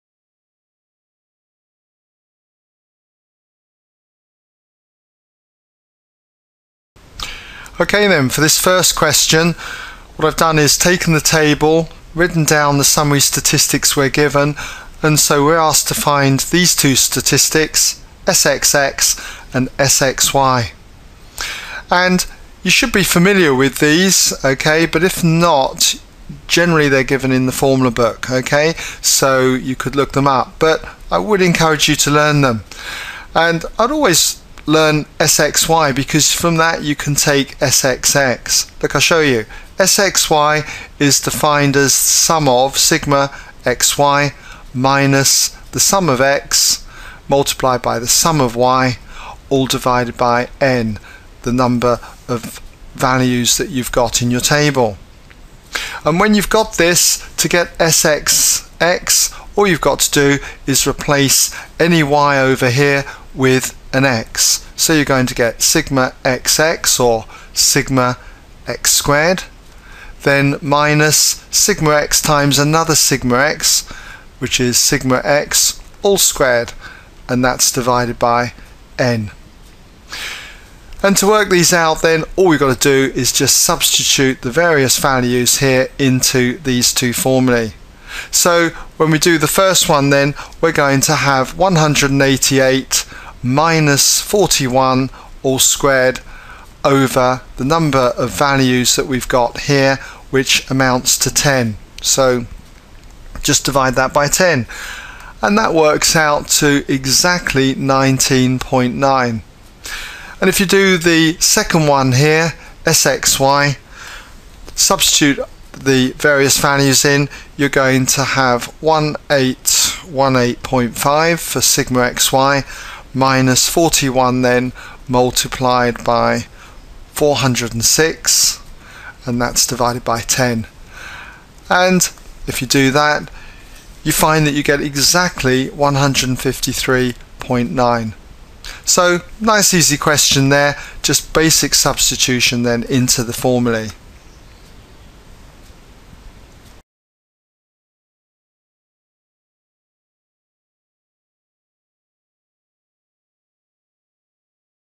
A-Level Edexcel Statistics S1 January 2008 Q4a (regression) : ExamSolutions
Worked solution to this question on regression. To see the question go to ExamSolutions